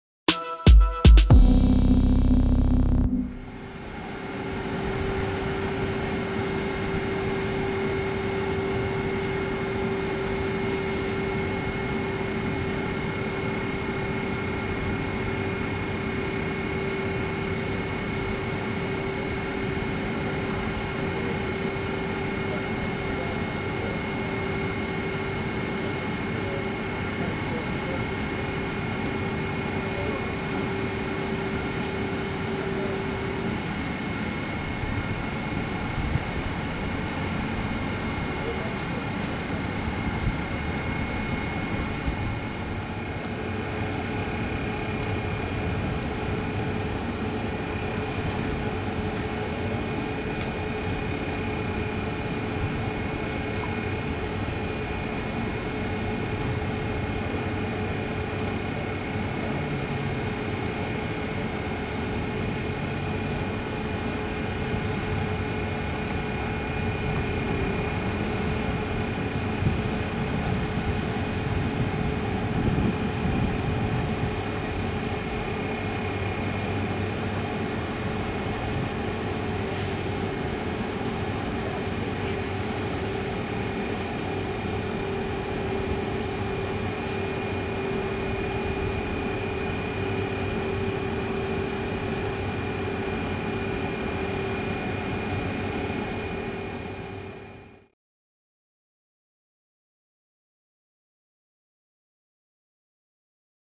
Taurus 1216 141 ÖAMTC in sosta nella stazione di Lienz
La Taurus 1216 141 in livrea speciale ÖAMTC è in sosta nella stazione di Lienz. Video di Lorenzo Pallotta.